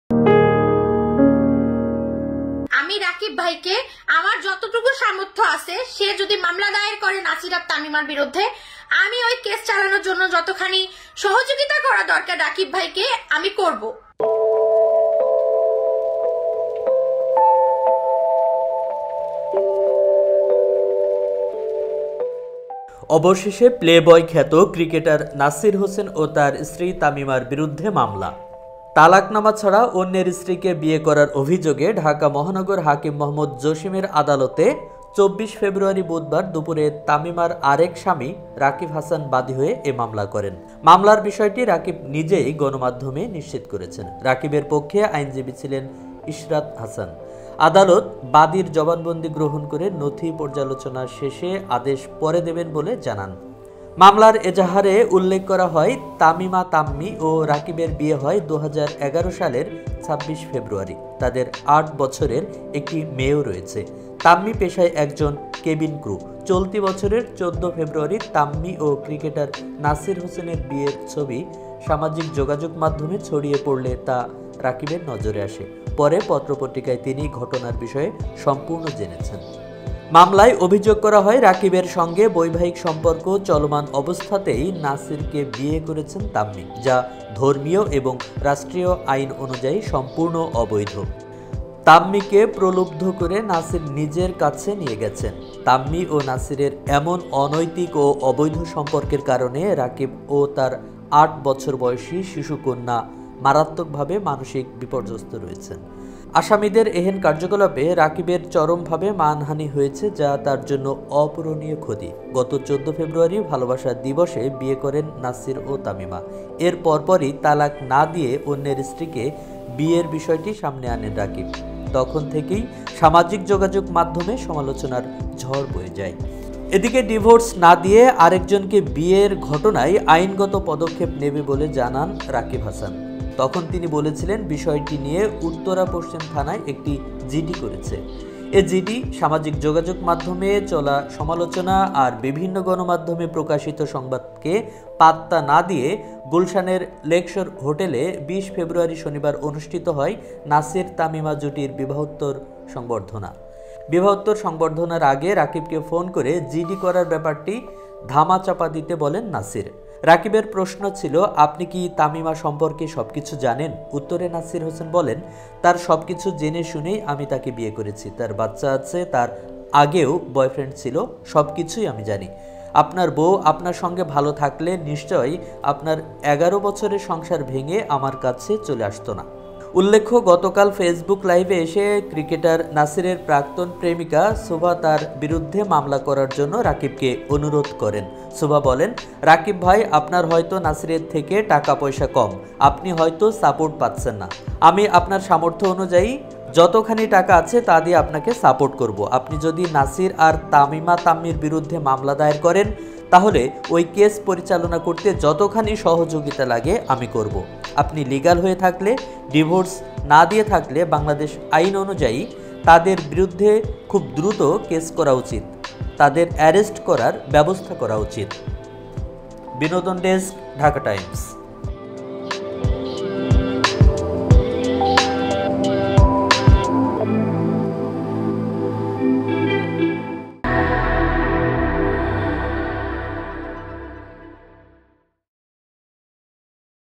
অবশেষে নাসির-তামিমার বিরুদ্ধে রাকিবের মামলা || Nasir & Timmi || Subha || Rakib || Dhakatimes
অন্যের স্ত্রীকে ডিভোর্স পেপার ছাড়াই বিয়ে করার অভিযোগে টাইগার ক্রিকেটার নাসির হোসেনের নামে মামলা করা হয়েছে। একই সঙ্গে মামলা করা হয়েছে নাসিরের স্ত্রী তামিমা তাম্মির বিরুদ্ধেও। আজ (বুধবার) ঢাকা মহানগর হাকিম মোহাম্মদ জসীমের আদালতে রাকিব হাসান বাদী হয়ে এ মামলা করেন।
ANTI-PIRACY WARNING
Protect by Dhaka Times Authority